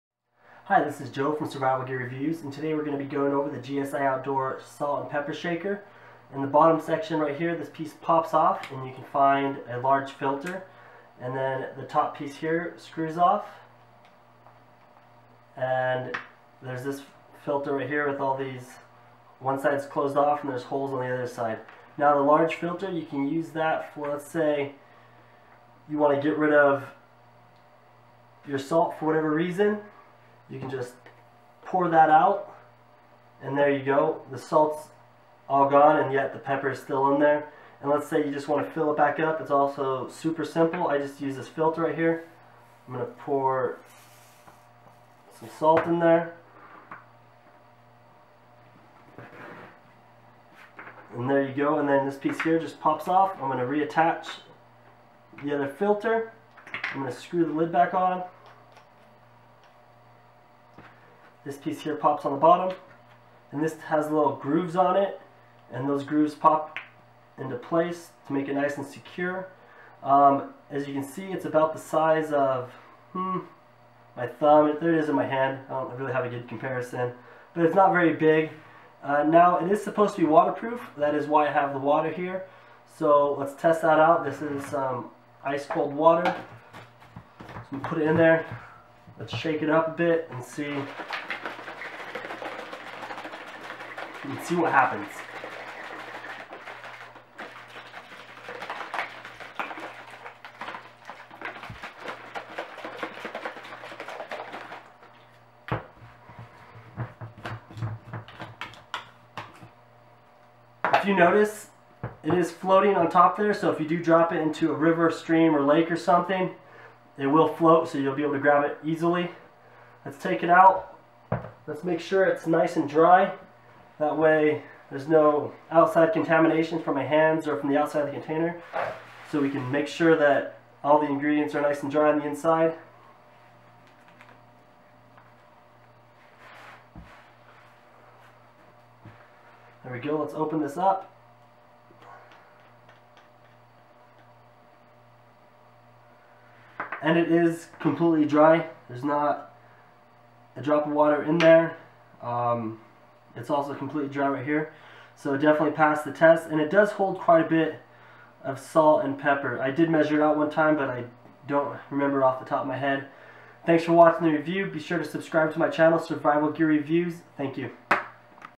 GSI Outdoors Salt and Pepper Shaker Review
A review and waterproof test of the GSI Outdoors Salt and Pepper Shaker.